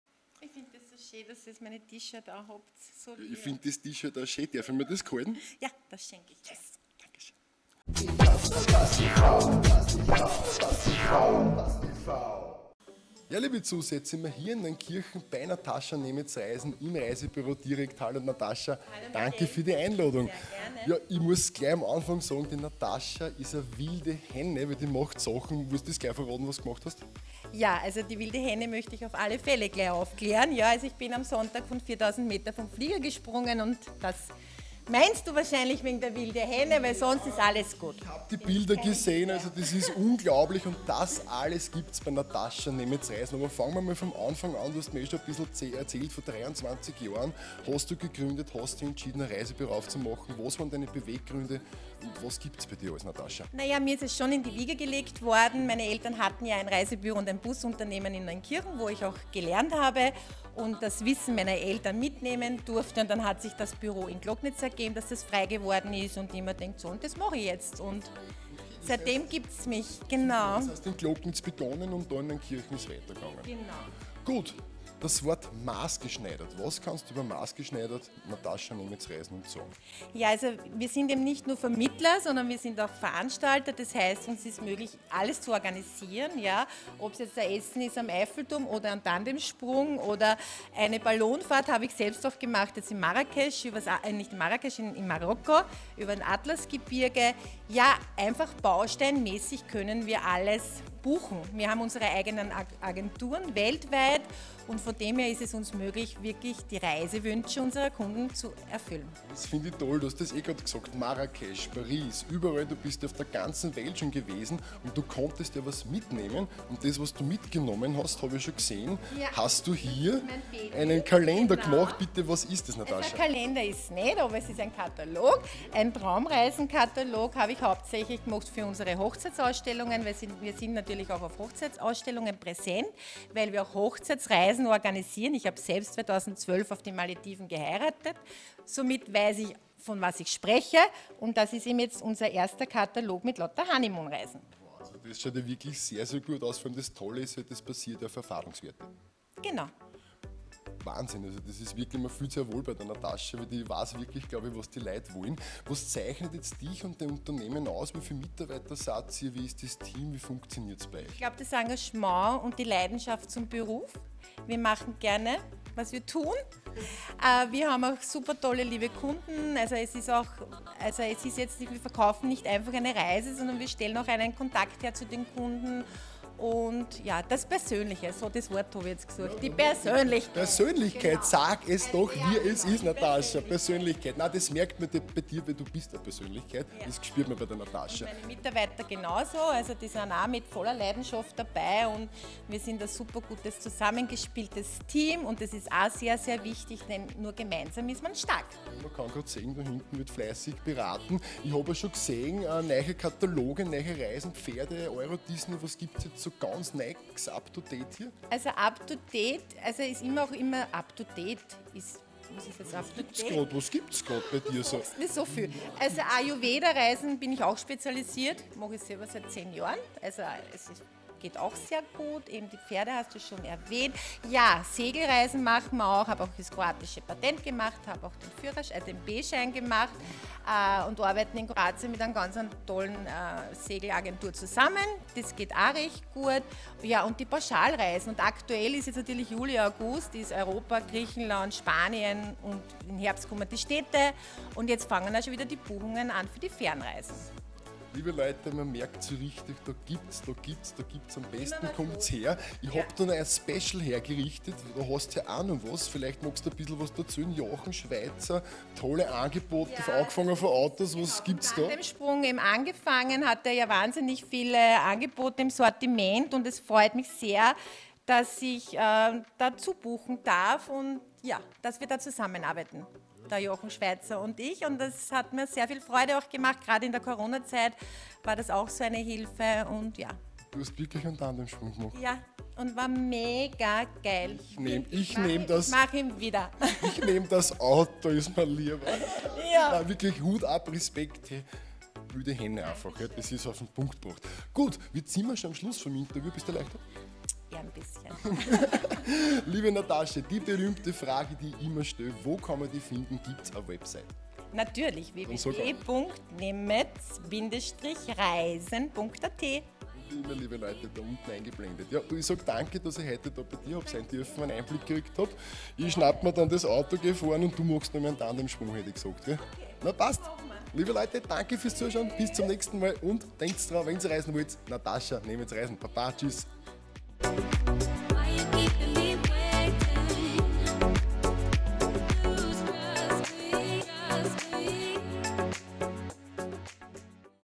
Zu Besuch bei Natascha Nemec Reisen
😃Wir waren in Neunkirchen und haben Natascha Nemec Reisen🌍 besucht. Wir sind absolut begeistert, tolle Angebote und Service. Aber das Beste ist das super Team dahinter. Was Natascha alles so erzählt, seht ihr im Video📺